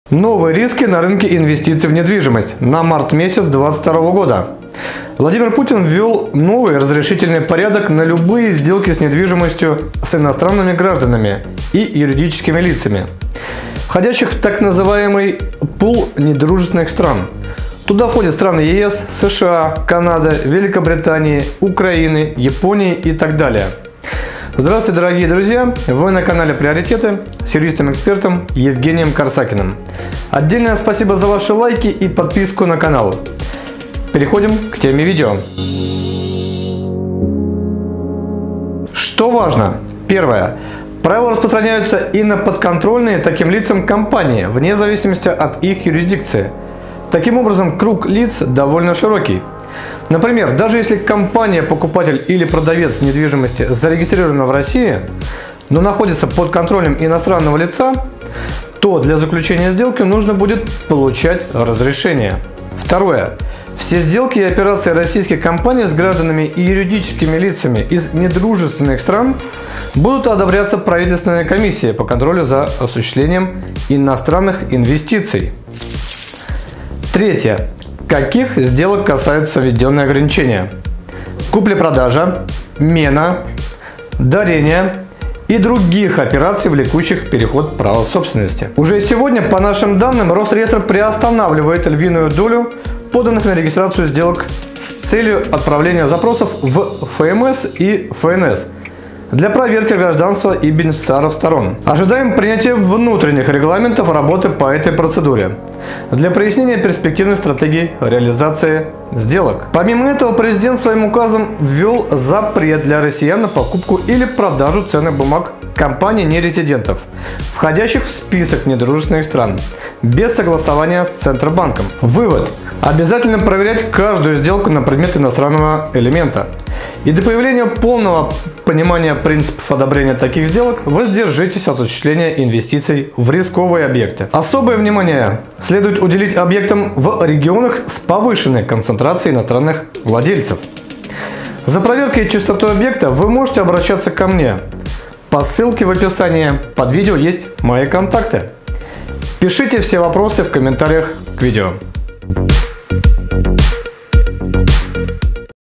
РЫНОК НЕДВИЖИМОСТИ 2022
★ В этом видео Мы разобрали: ⚡️Новые риски на рынке инвестиций в недвижимость на март месяц 2022 года
★ Владимир Путин ввел новый разрешительный порядок на любые сделки с недвижимостью с иностранными гражданами и юридическими лицами, входящие в пул так называемых «недружественных стран»  (туда входят страны ЕС, США, Канада, Великобритания, Украина, Япония и т.д.).